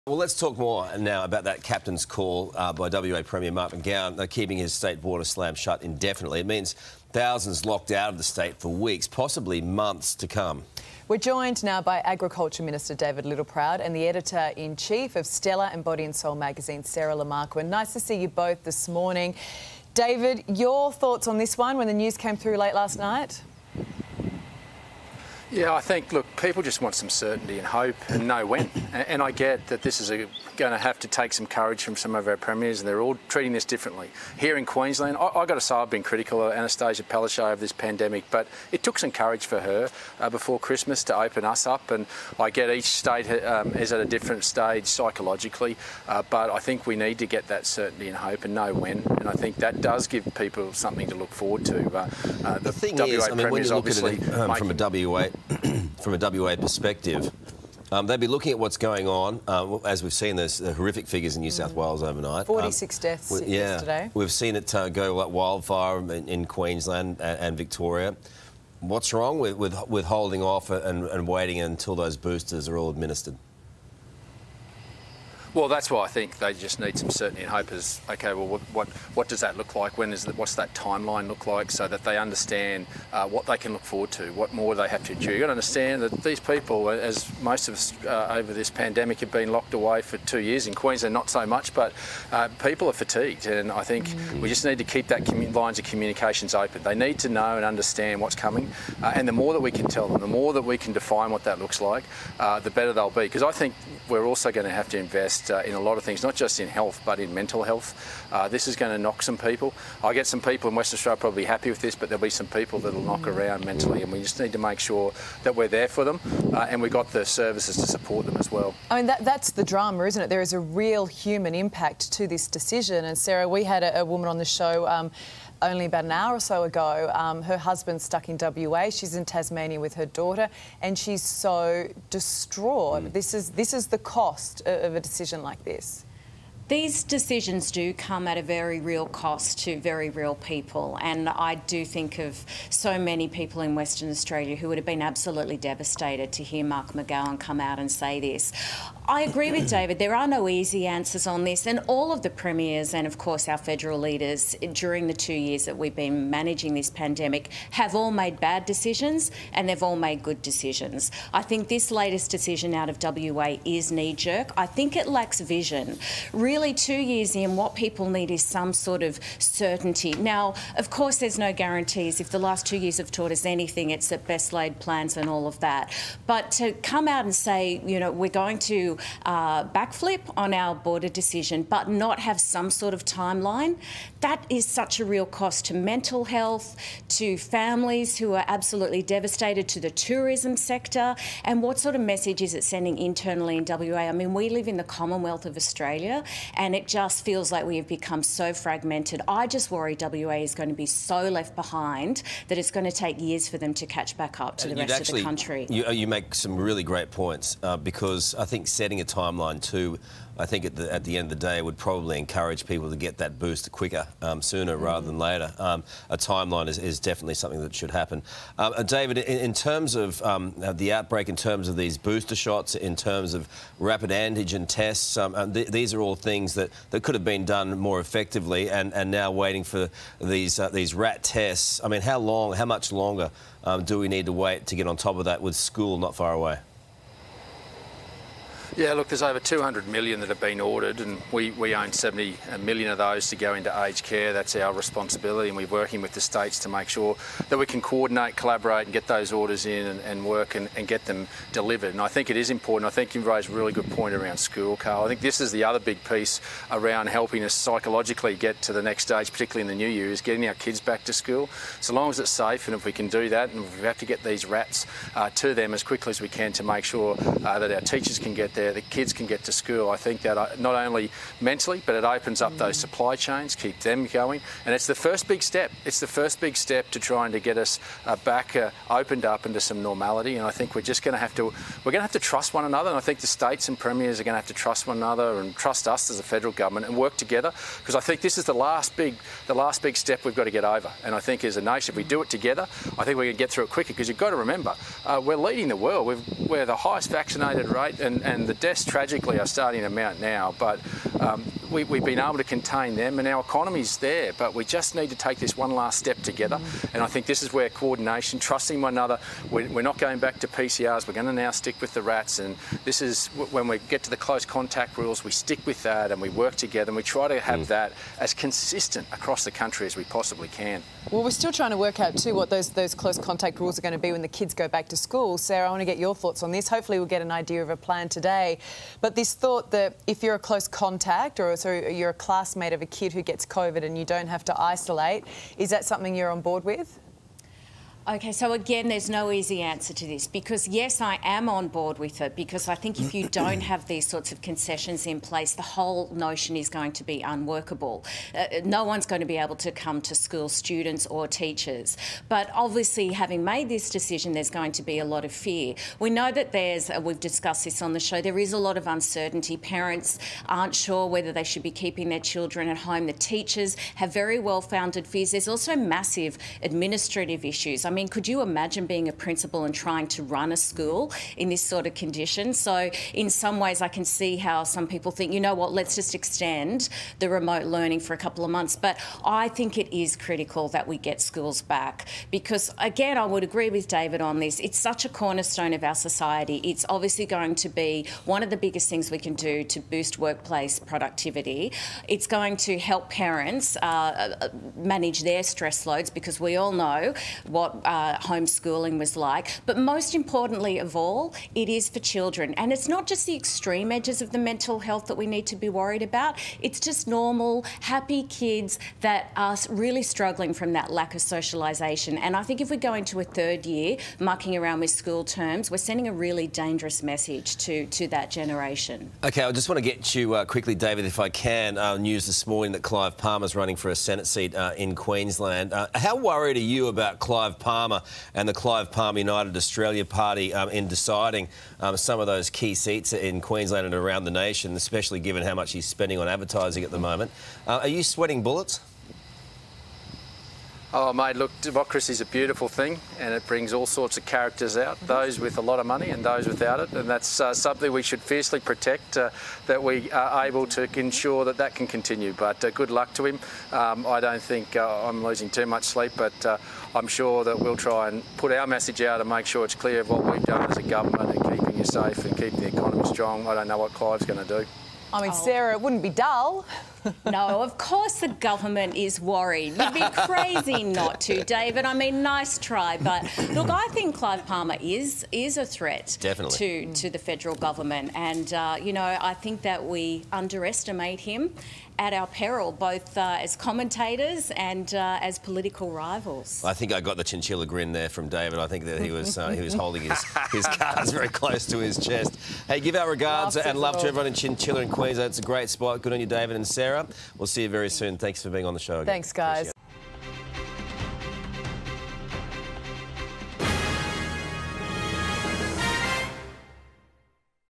‘People are fatigued’ after WA border backflip | Coronavirus | 9 News Australia
Agricultural Minister David Littleproud and Stellar editor-in-chief Sarrah Le Marquand discuss the impact on 'exhausted' Aussies, after Premier Mark McGowan's call to keep the Western Australian border shut as COVID-19 cases continue to surge in the country.